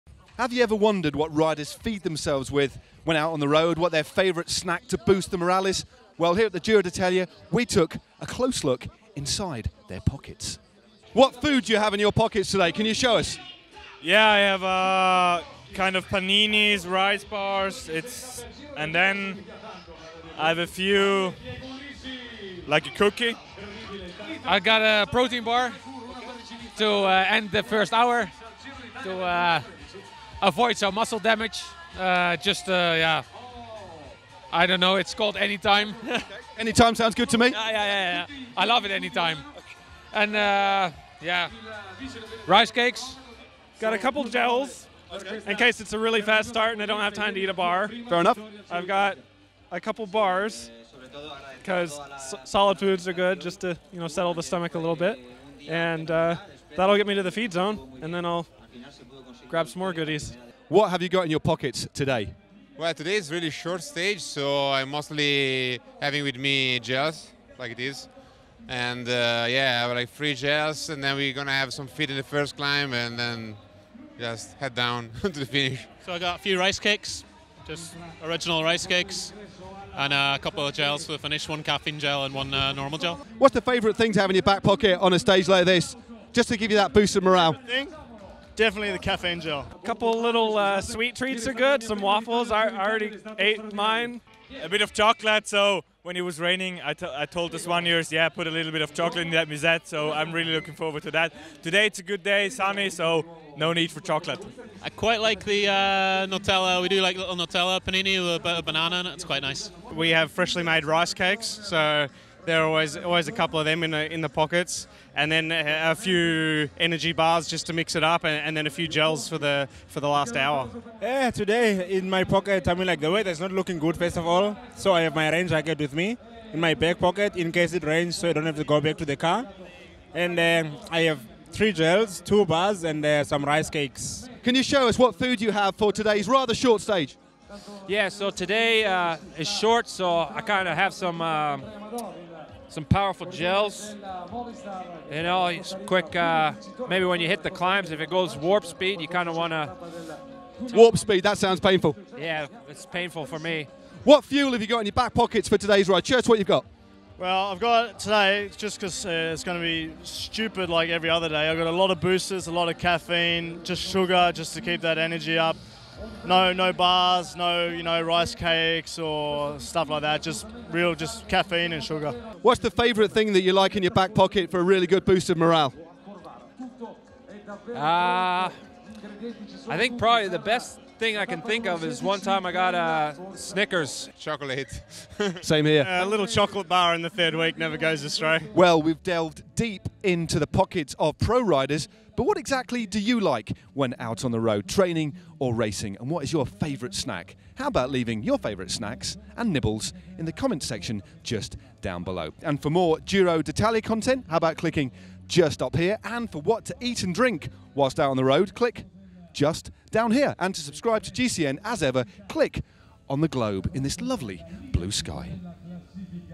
What's In Your Pockets?! How Do Pro Cyclists Fuel For A Stage At The Giro D'Italia 2016?
"That'll get me to the feed zone, and then, well... I'll grab some more goodies..."

What do the pro peloton like to eat during a stage of the Giro d'Italia? It's not all energy gels and bars.

What should you eat and drink when you're out riding?